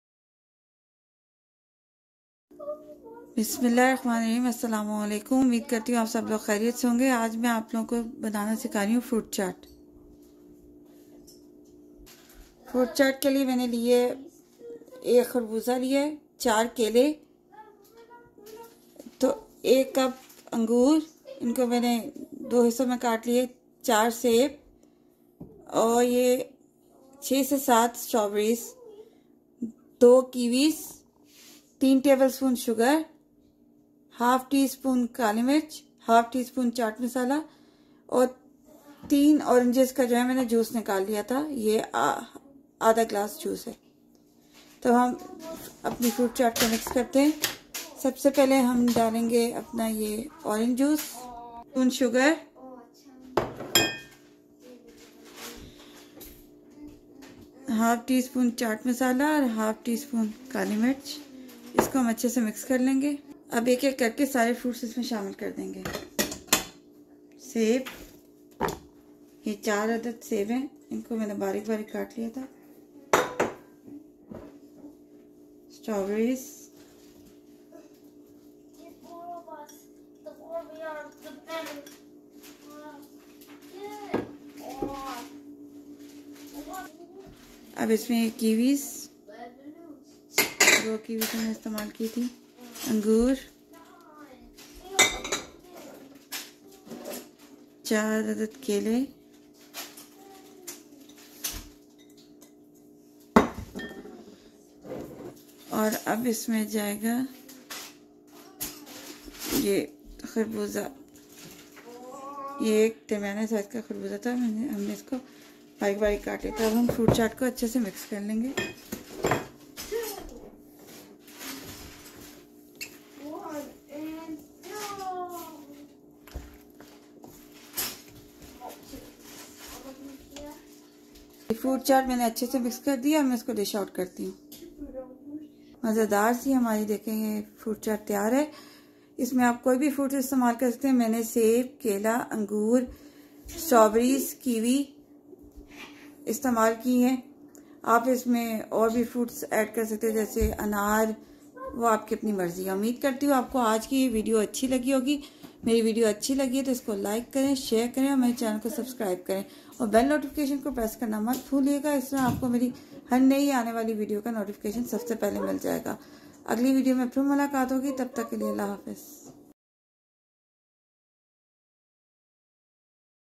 Sweet and Spicy Fruit Chaat ( Fruit Salad)
Fruit salad is a dish consisting of various kinds of fruit, sometimes served in a liquid, either their own juices or a syrup